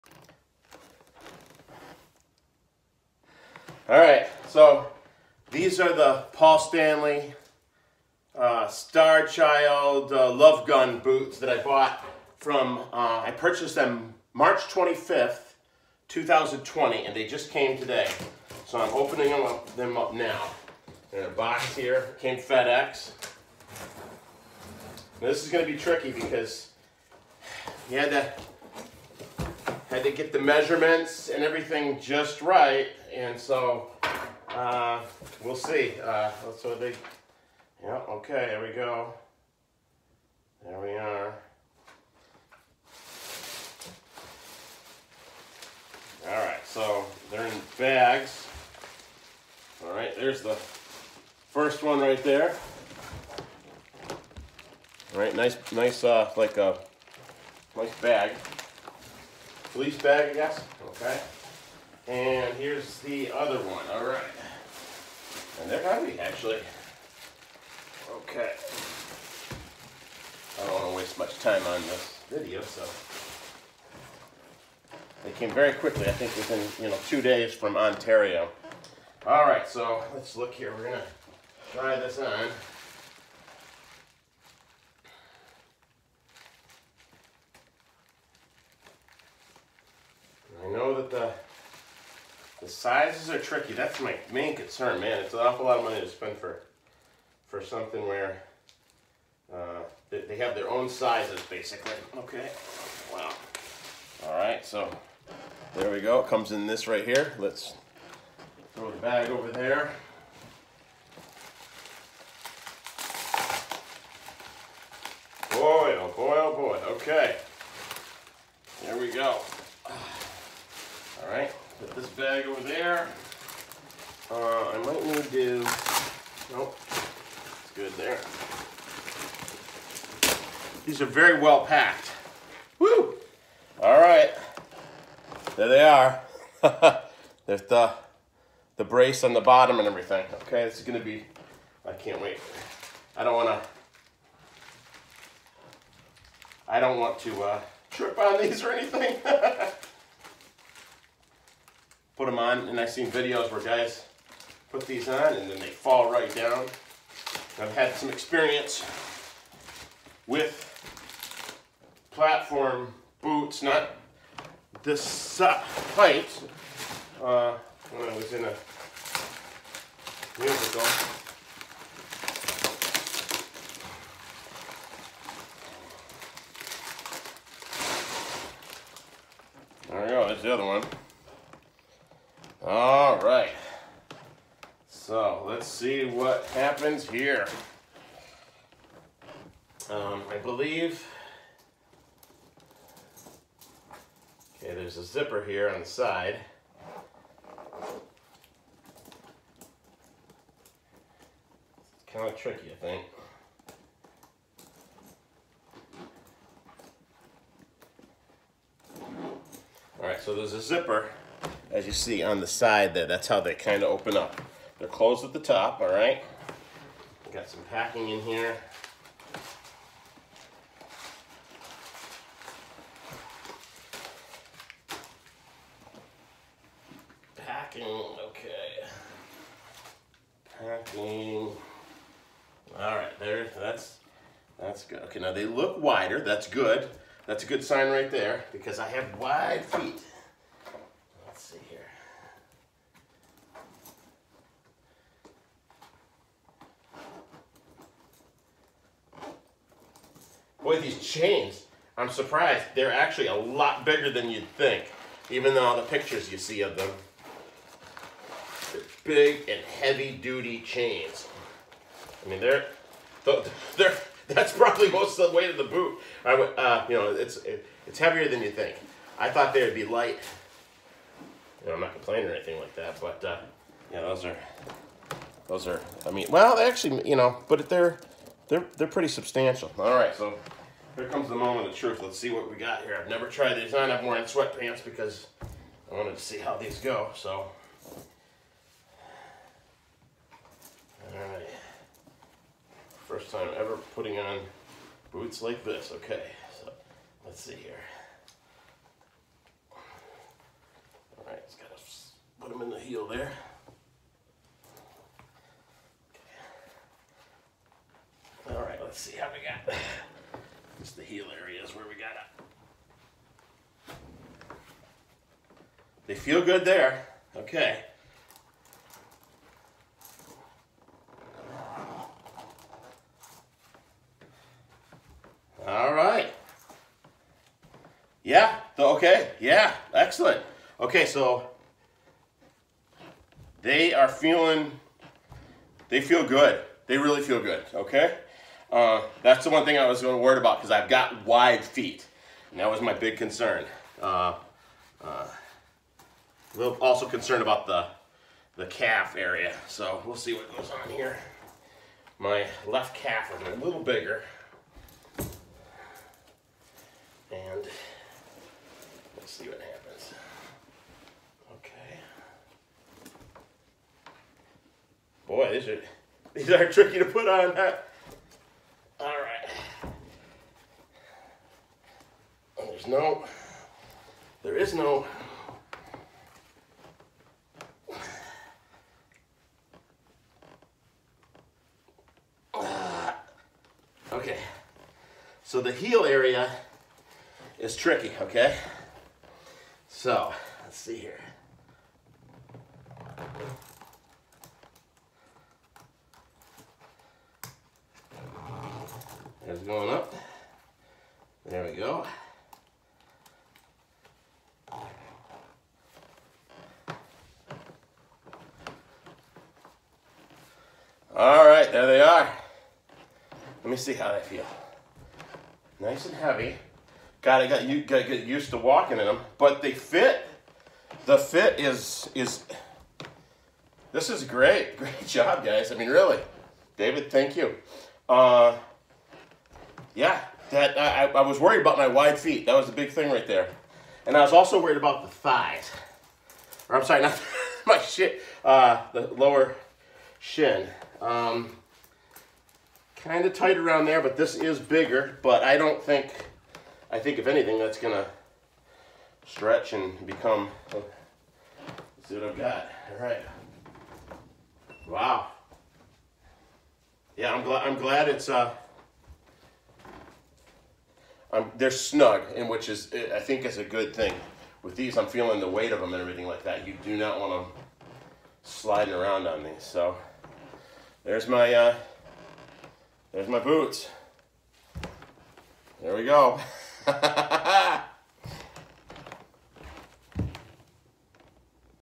Trying on KISS Replicas Starchild Love Gun Boots first time.
A quick unboxing and review!  Will they fit correctly?  Watch to find out!